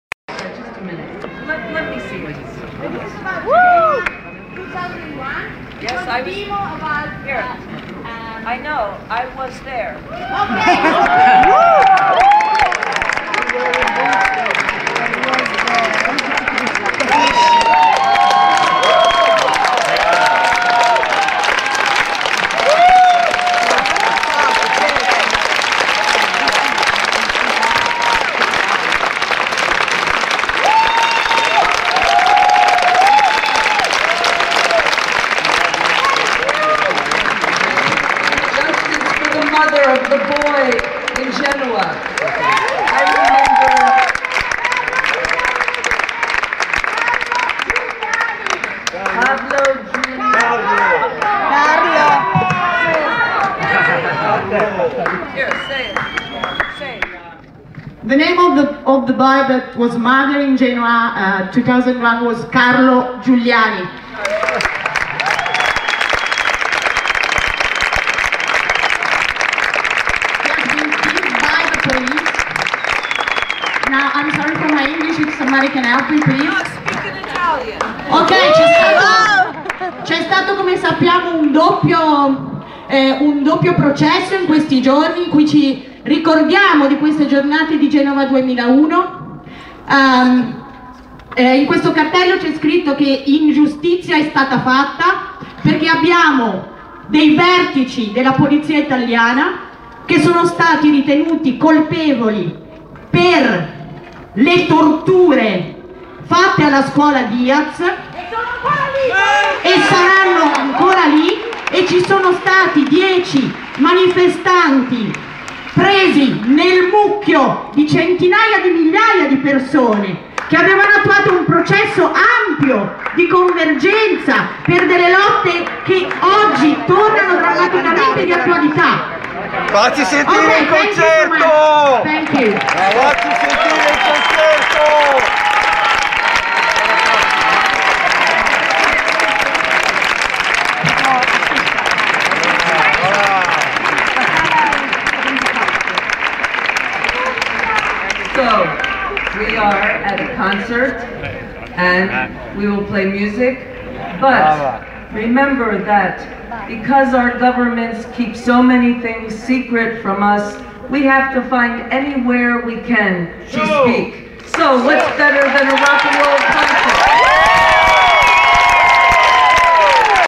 Patti Smith ricorda Genova 2001 e Carlo Giuliani, Bologna, 15/07/2012
Due giorni dopo la sentenza della Cassazione nota come "sentenza 10x100", Patti Smith è a Bologna per il "Concerto per la memoria di Ustica", Bologna. Attivisti mostrano due cartelli con le scritte "GENOVA 2001" e "INGIUSTIZIA E' STATA FATTA", Patti li mostra e consegna a un'attivista il microfono per un intervento. Applausi. Soltanto un coglione grida: "Facci sentire il concerto!", ma l'artista interviene di persona e lo cazzia, ricordandogli che la musica non è avulsa dal mondo. Del resto, si era lì per ricordare la strage di Ustica! Se non ti va bene, stai a casa tua, no? Parliamo di Patti Smith, mica di Valerio Scanu.